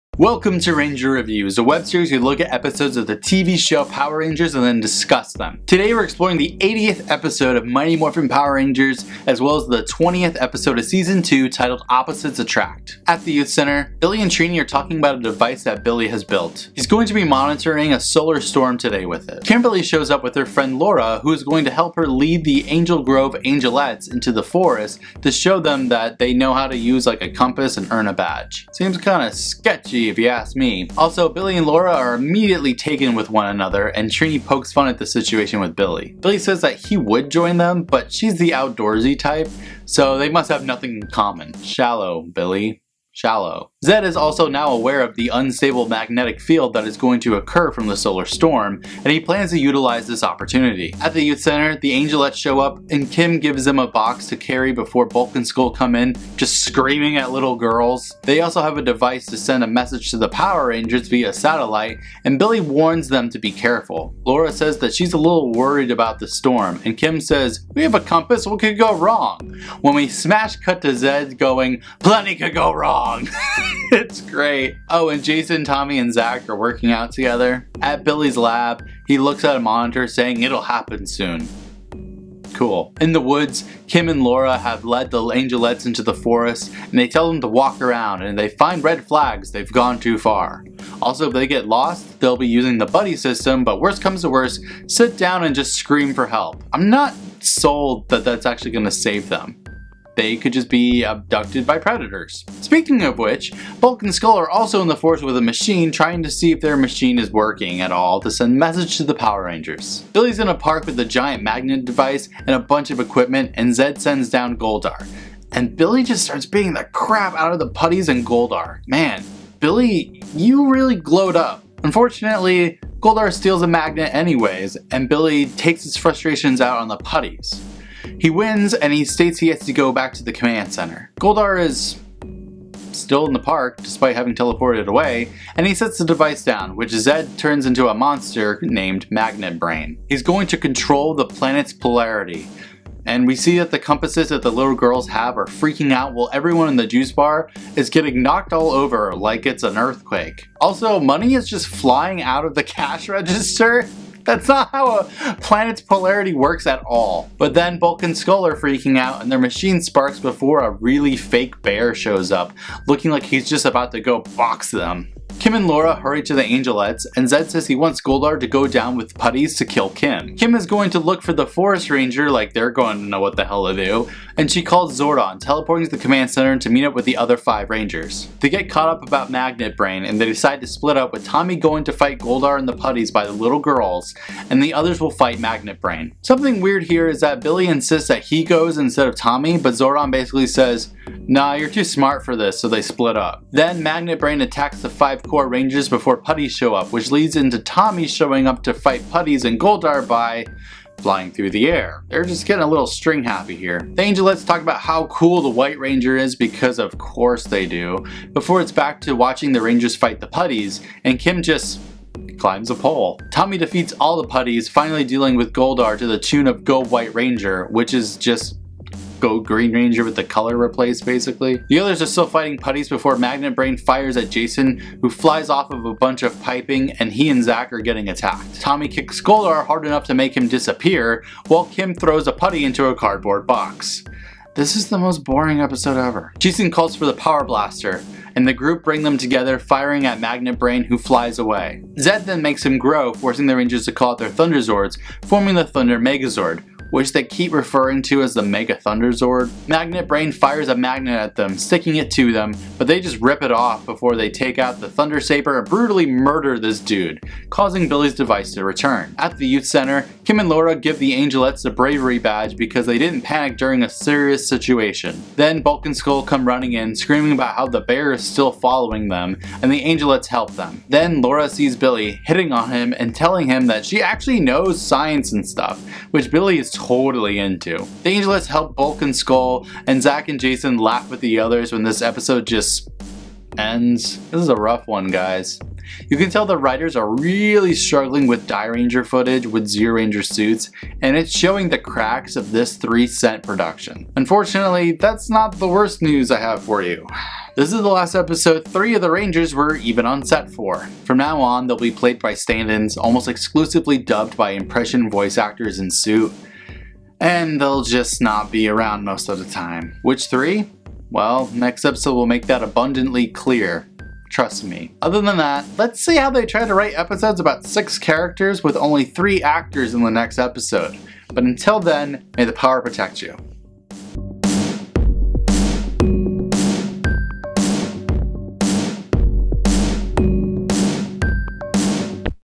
Mighty Morphin Power Rangers - S02E20 - Episode Review
Welcome back to Ranger Reviews, a series where we watch an episode of Power Rangers and discuss it! We keep it going with Mighty Morphin Power Rangers, Season 2 Episode 20: Opposites Attract.

Equipment -
Camera: Canon Vixia HF R52
Microphone: Røde VideoMic
Lights: Cowboy Studio Softbox Lightning x 2 (600 Watt)
Editing: Final Cut Pro, Adobe After Effects